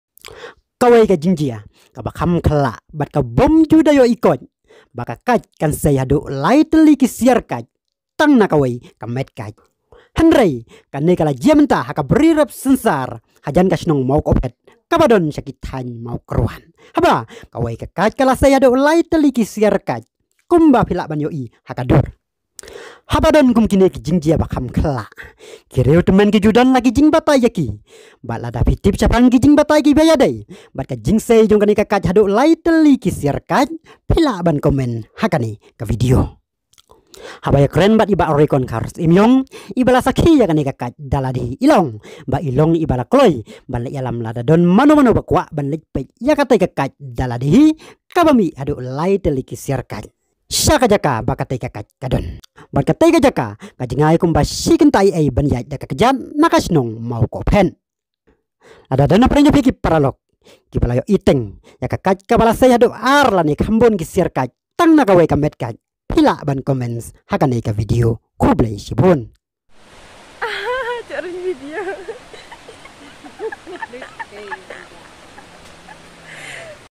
Ka jingjia ba khyllah shisha ; Sei ka kait haduh lai tylli ki Siar Kait . Mano bala ju iohi teng???
In this video we show you about an extra Ordinary Banana tree which blooms with three flowers.  This happened in South West Khasi Hills District of Meghalaya near the village Mawkohphet.